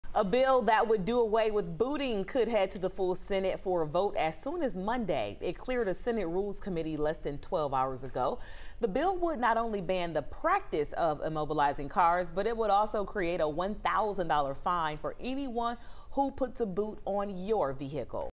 Bill that would ban booting in Georgia moves forward in state Senate
The bill would also create a fine for applying boot devices.